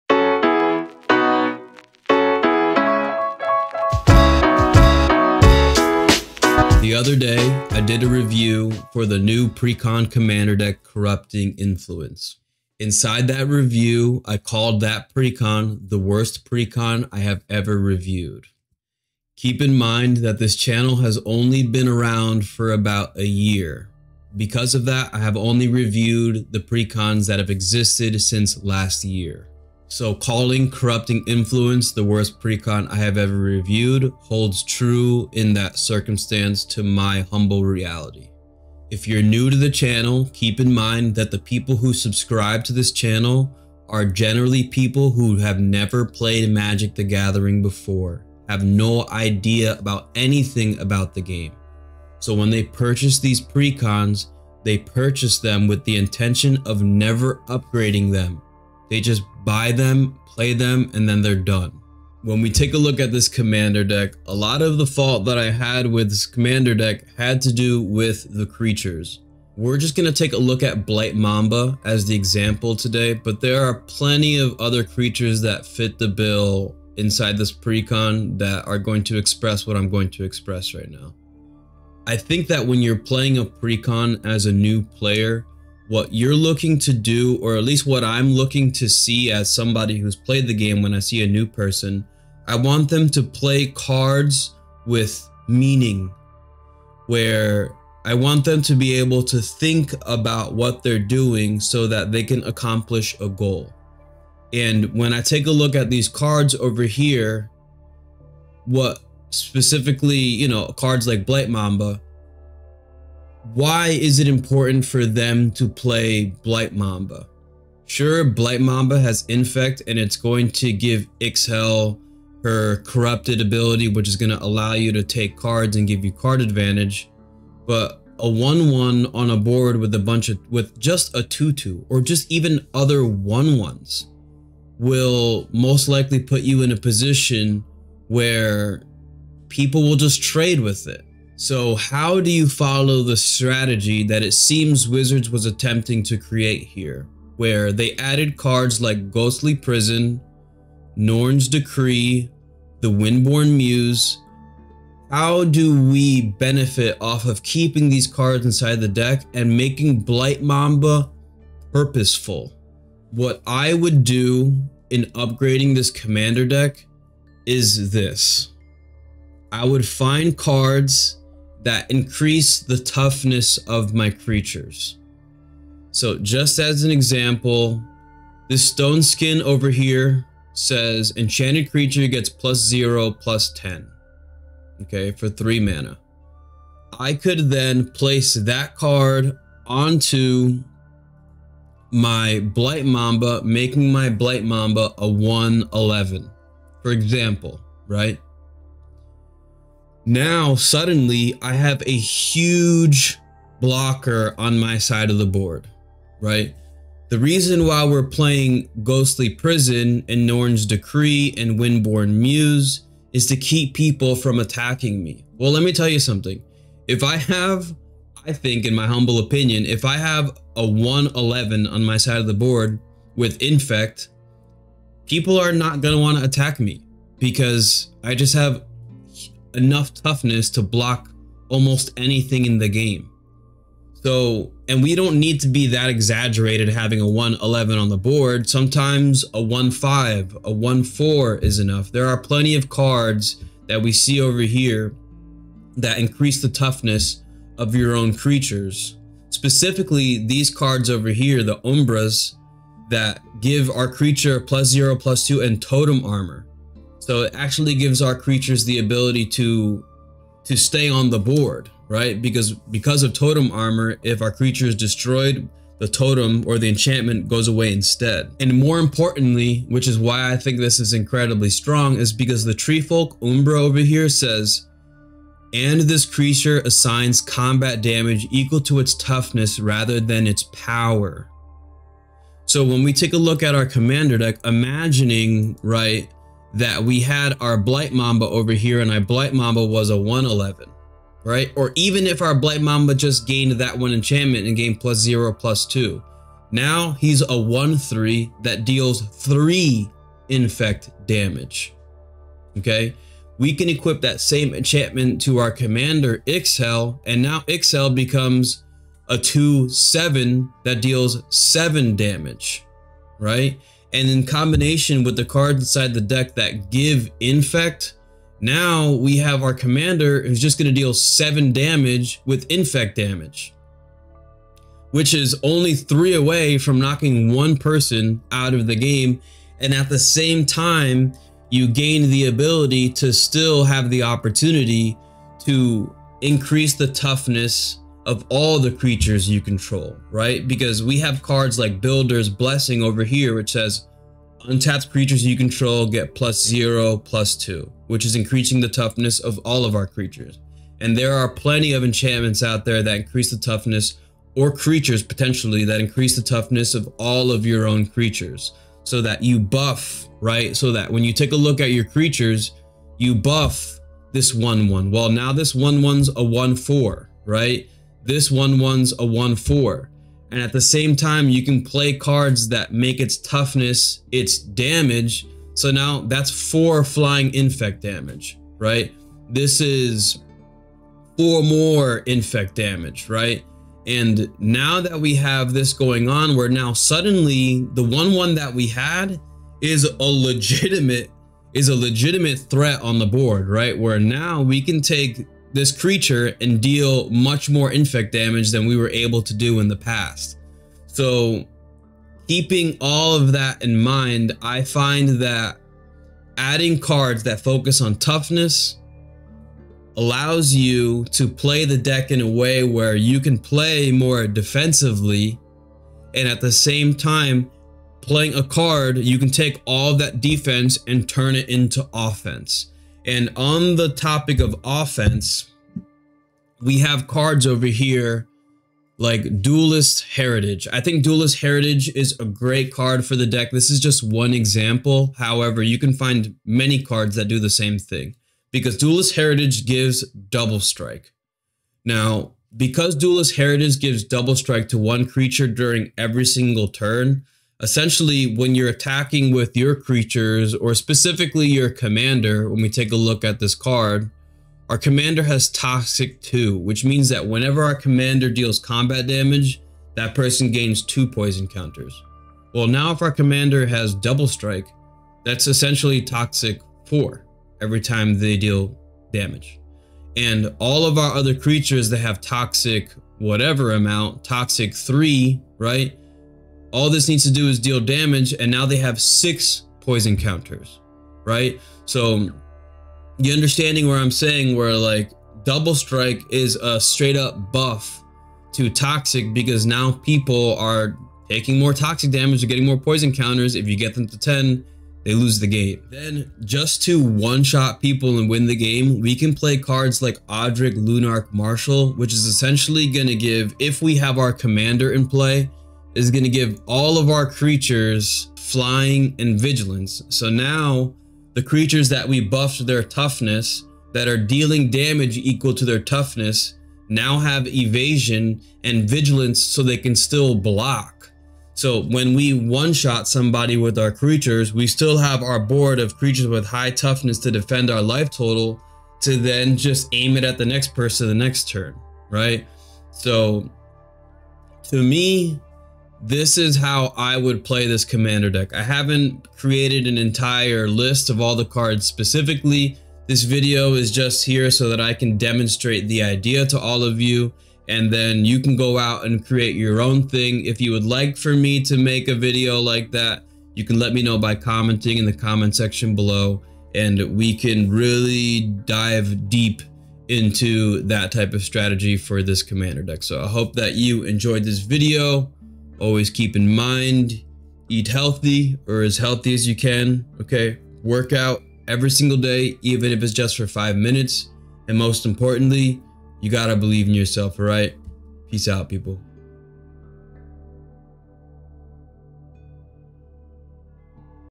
Upgrade Strategy Guide Corrupting Influence Precon | OTK Infect Pillowfort | Ixhel, Scion of Atraxa
Phyrexia All Will Be One Magic the Gathering Commander EDH MTG

My name is Kevin, and I am grateful to have you here.